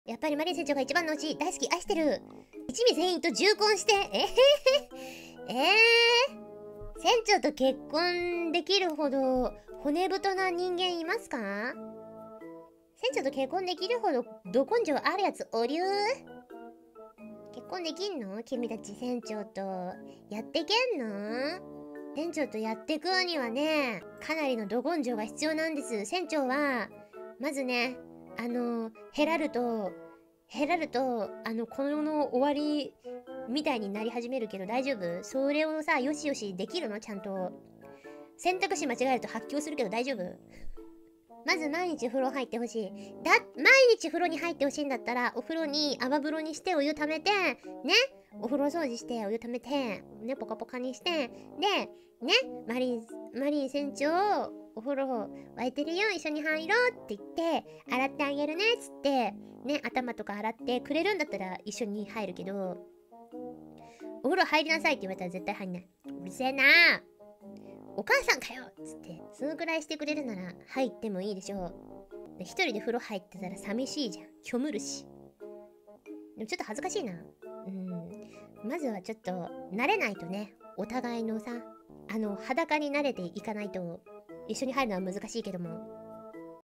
【Eng Sub】What Senchou wants her marriage partner to do? Part 3【Houshou Marine / hololive】
This clip has English subtitles as well as additional explanations of the Japanese meanings.
All explanations of the Japanese meanings shown in this clip are intended to make the Japanese
language easier to understand for English speakers.